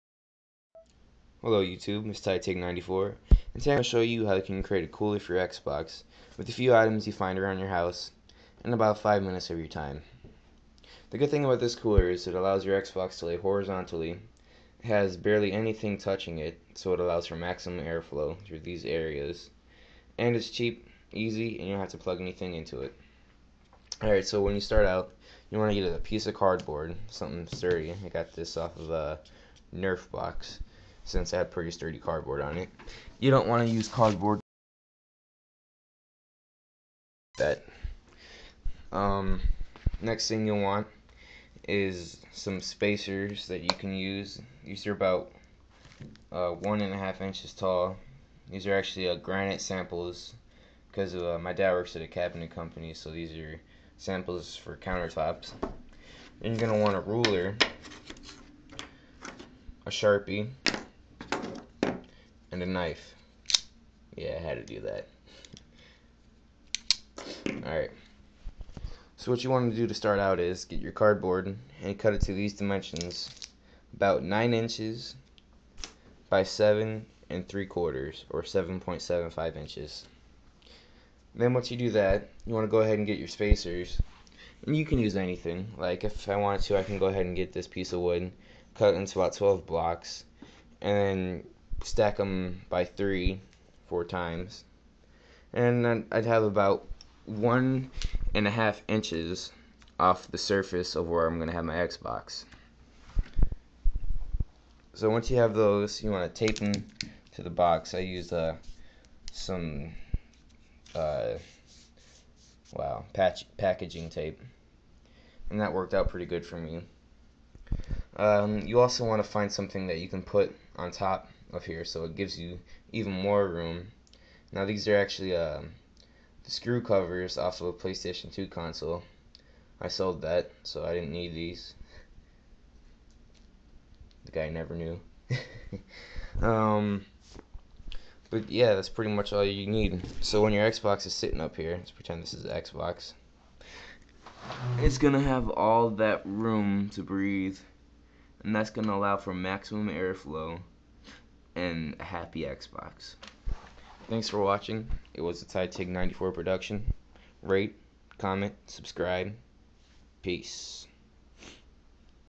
How to Make a Xbox 360 Cooling System for Free
Today I show you how to create a xbox 360 cooling system with items you can find around your house.